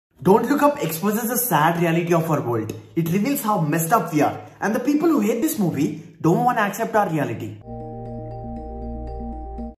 Reviewing Don't Look Up in 10 seconds or less
In 10 seconds or less, this is my review of Netflix's Don't Look Up.

If you have watched it, let me know what you think about it in the comments.